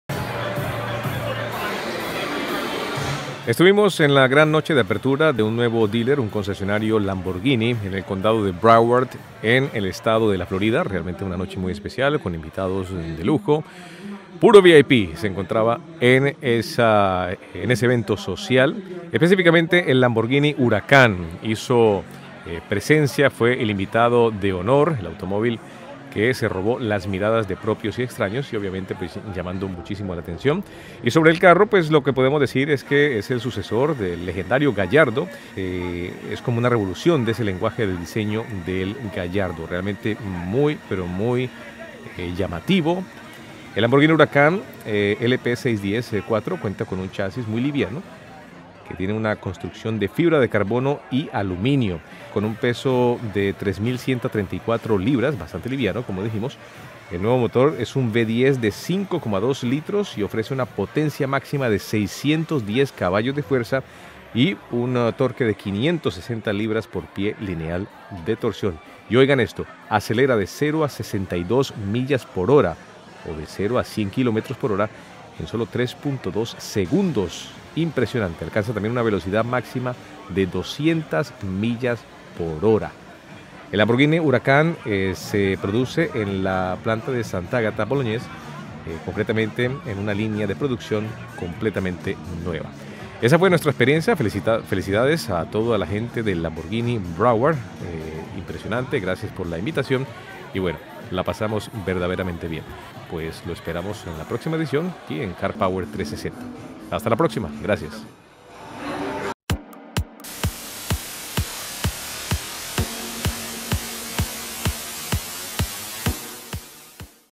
Lamborghini Huracan | en Español | Spanish - Presentación especial
Estuve en la noche de apertura del nuevo dealer Lamborghini Broward en el Sur de la Florida y allí fue presentado el nuevo Lamborghini Huracan 2016. La versión Lamborghini Huracan Spyder también estaba en exhibición. Esta no es la prueba Lamborghini Huracan en español, es solo una presentación especial del vehículo en el concesionario. Aquí tienen un pequeño vistazo a este superdeportivo de la marca italiana.